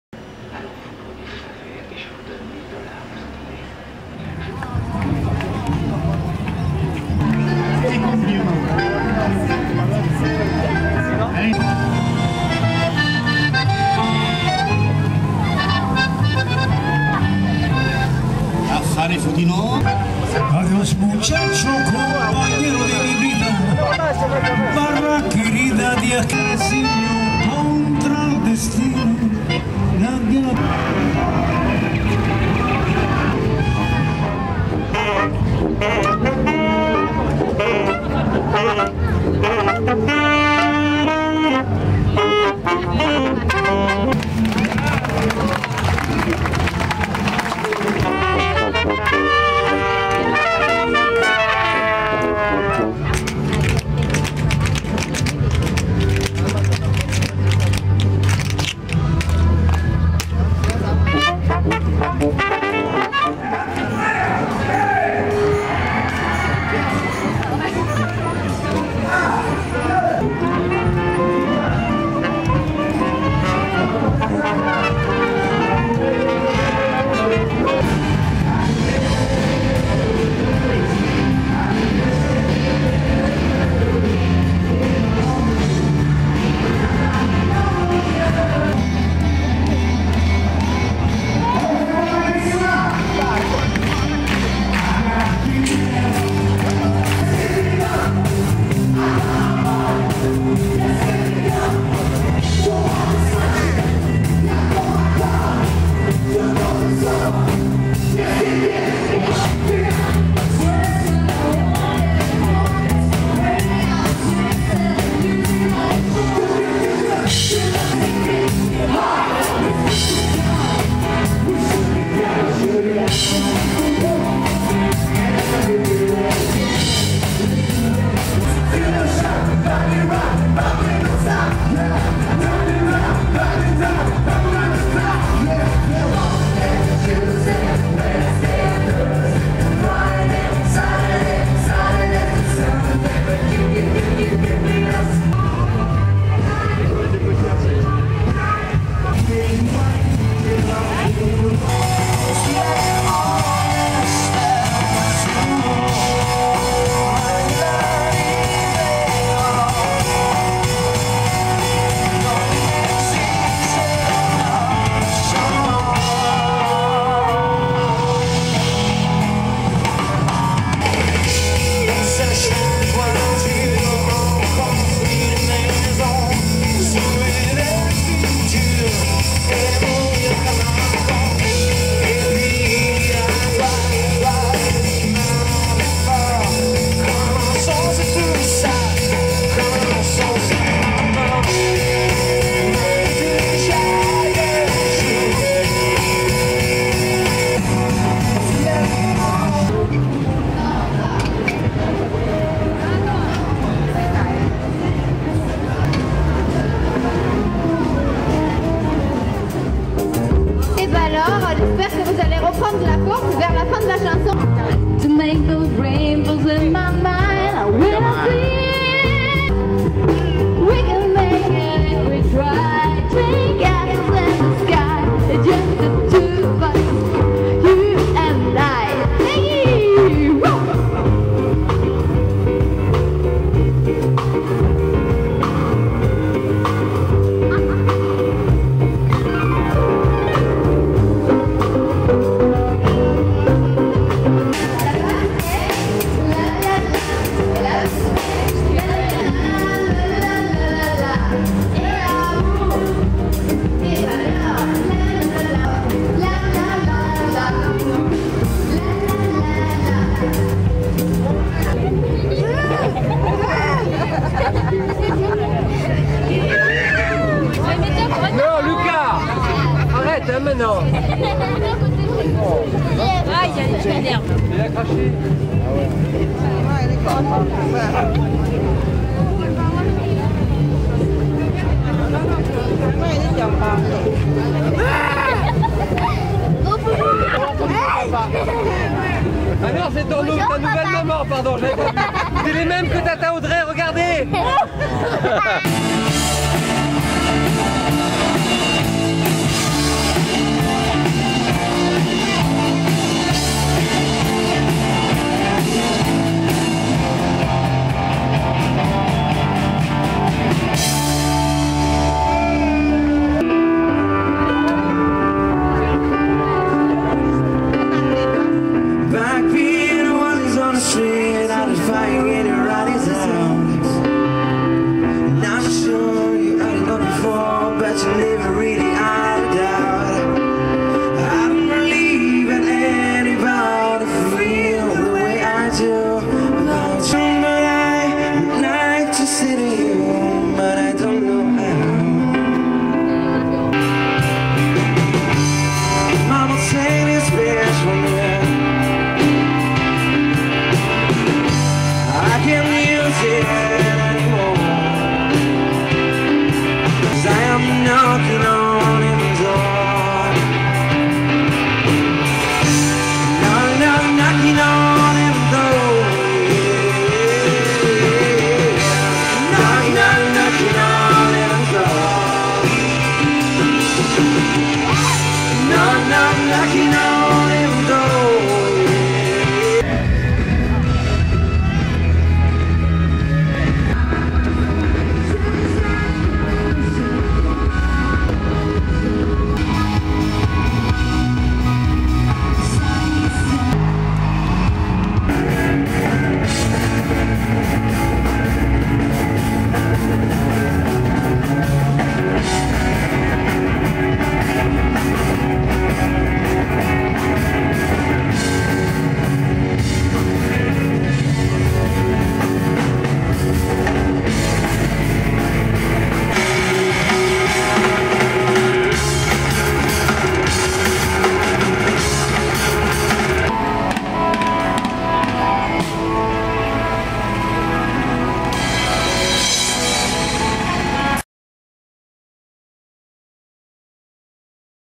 nice : prom' party juillet 2010 par tvs (lao-tvs video)
la première prom'party à nice une soirée déferlante de bonne ambiance avec plusieurs groupes sur podiums mobiles.video realisée par tvs